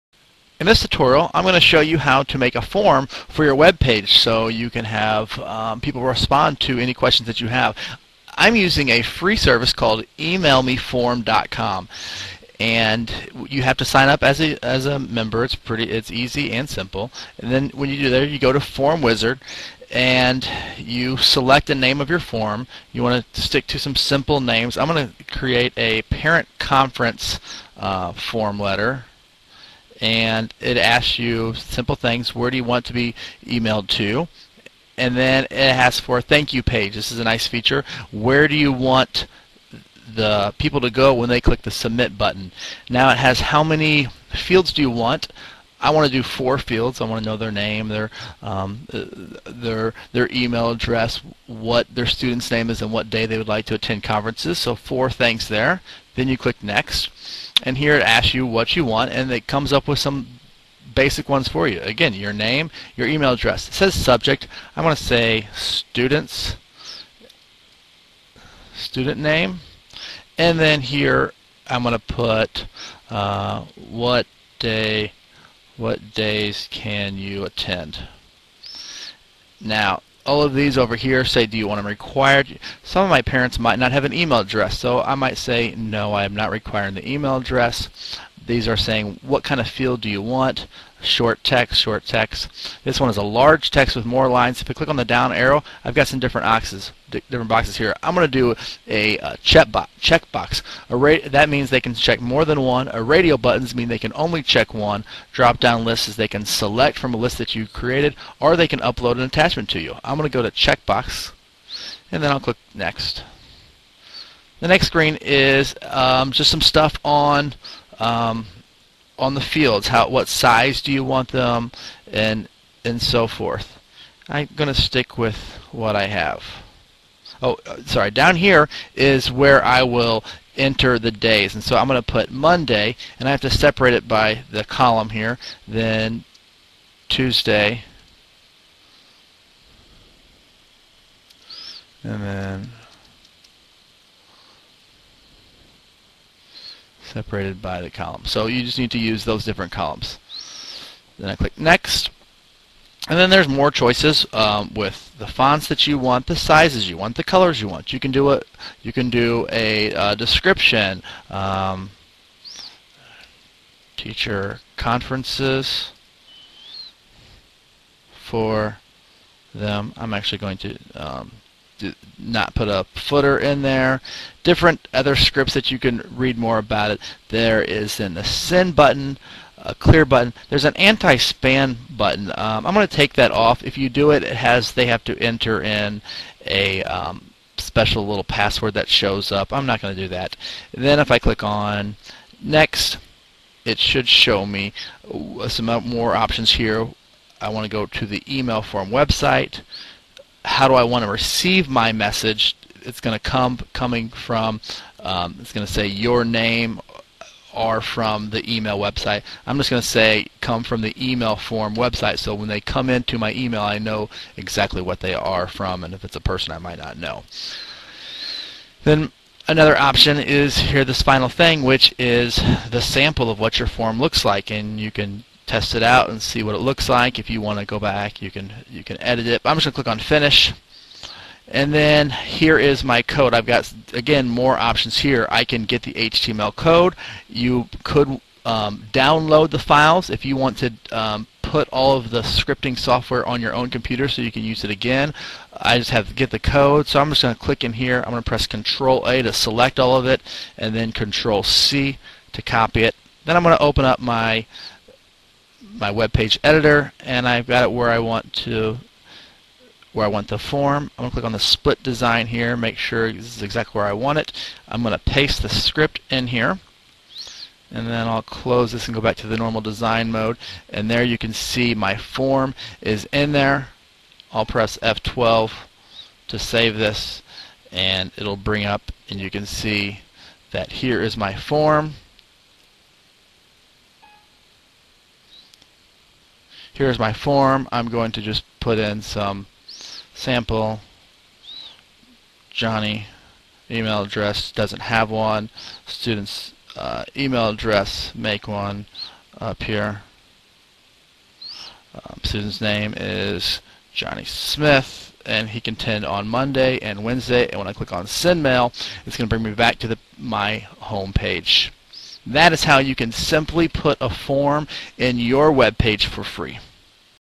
Add a mail form to your webpage for free
This video shows how to add a form to your webpage so your viewers can submit a message, complete a sign-up form, request information, or even send an attachment that you will receive through your email. The form is free to use from EmailMeForm.com after you signup. The video also explains how to download the script and insert it into Dreamweaver.